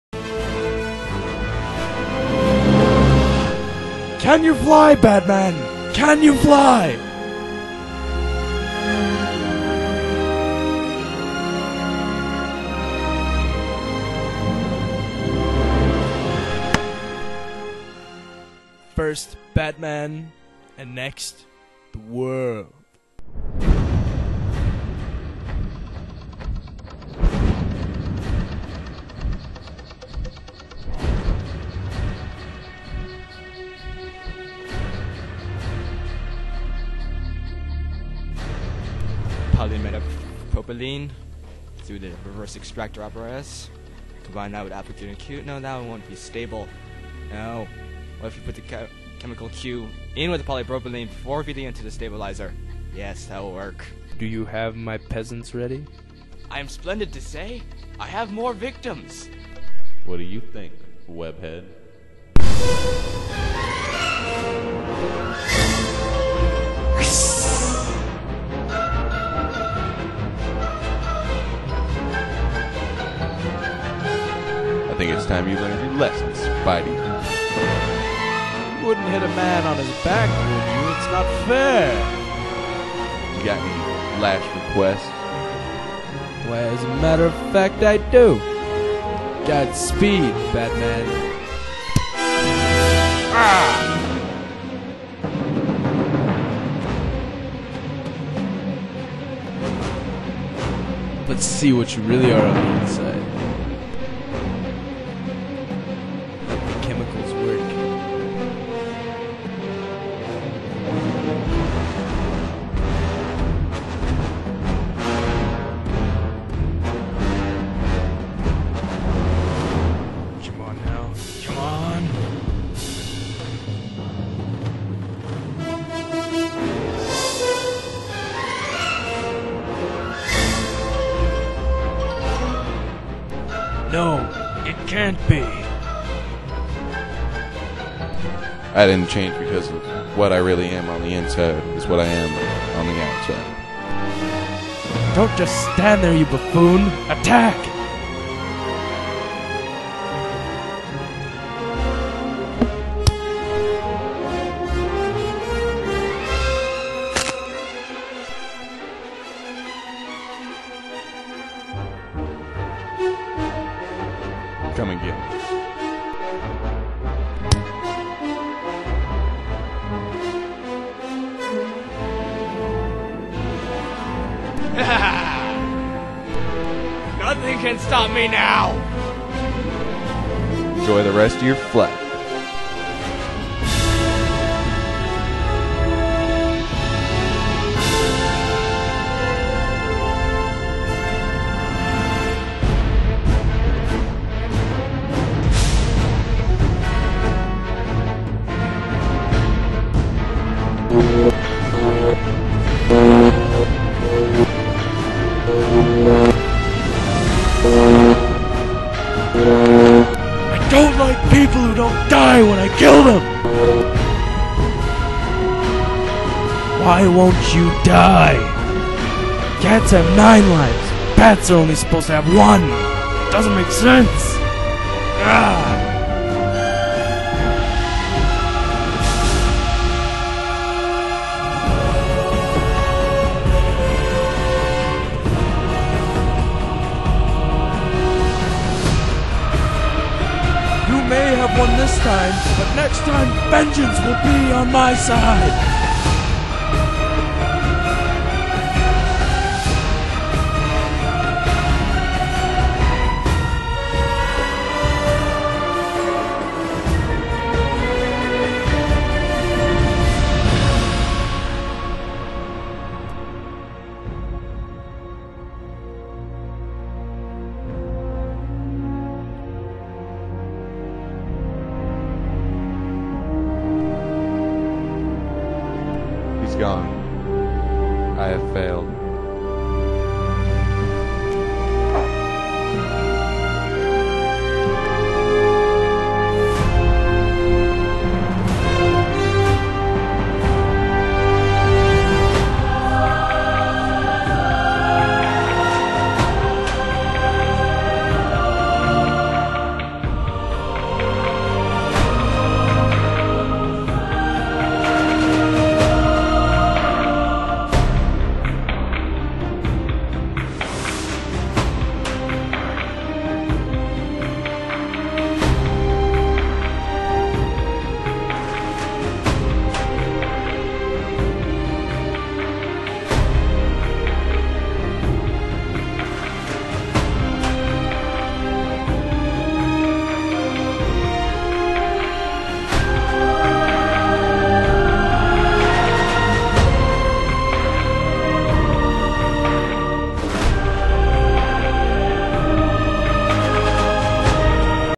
Gotham Havoc Part 3 (Batman film)
Part 3 of Gotham Havoc. Mutants appear to be kidnapping the people of Gotham and using them on their experiments. However, Batman fears that something much larger is in works.